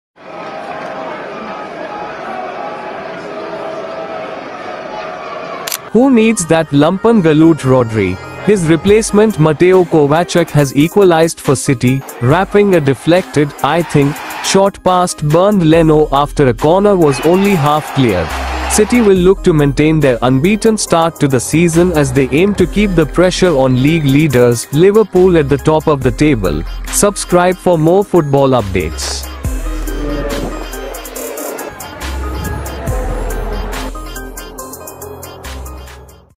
Mateo Kovacic INSANE GOAL For Manchester City vs Fulham
Mateo Kovacic Scores INSANE GOAL For Manchester City vs Fulham

Who needs that lumpen galoot Rodri? His replacement Mateo Kovacic has equalised for City, rapping a deflected (I think) shot past Bernd Leno after a corner was only half cleared.City will look to maintain their unbeaten start to the season as they aim to keep the pressure on league leaders Liverpool at the top of the table.